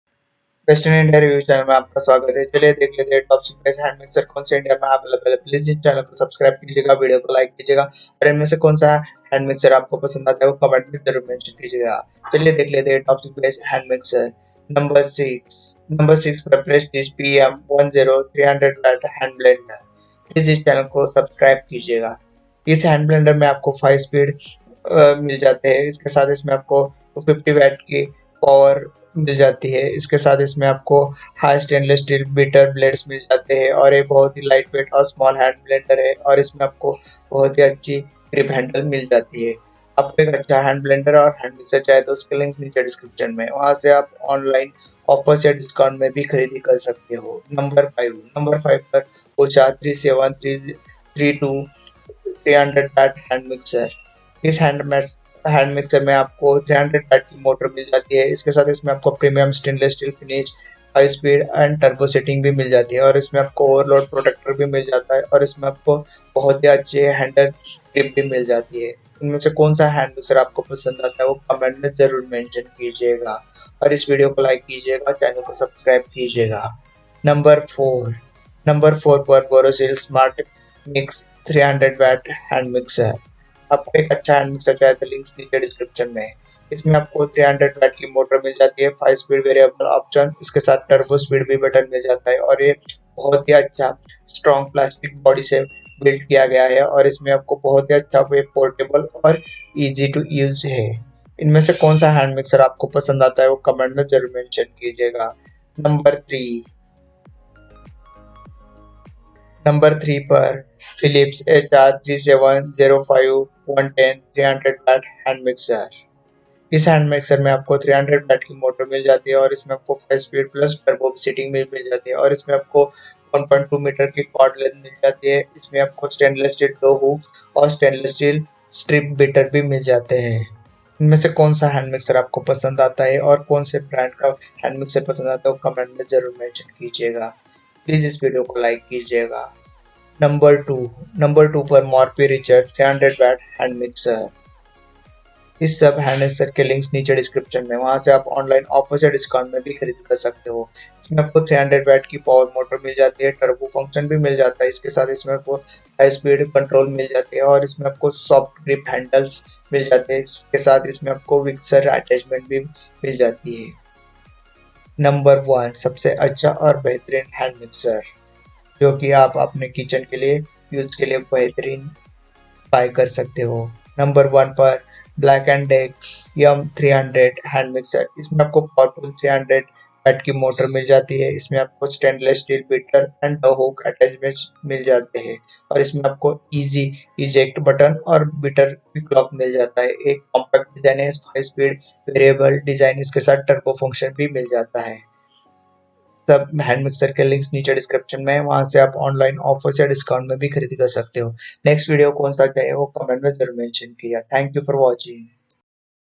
Best Hand Mixer in India 2023 ⚡ BEST HAND BEATER ⚡ हैंड मिक्सर ⚡ हैंड ब्लेंडर मशीन
Best Hand Mixer in India  ⚡ हैंड मिक्सर ⚡ हैंड ब्लेंडर मशीन

List of Top 6 Best Hand Mixer in India 2023

1. Black+Decker M300 300-Watt Hand Mixer

2. Morphy Richards 300-Watt Hand Mixer

3. Philips HR3705/10 300 Watt Hand Mixer

4. Borosil Smartmix 300-Watt Hand Mixer

5. Usha 3732 300-Watt Hand Mixer

6. Prestige PHM 1.0 300-Watt Hand Blender

Checkout the list of Top 6 Best Hand Mixer in India.
Which watt hand mixer is best?
Which hand cake mixer is best?
Which hand blender is best for home?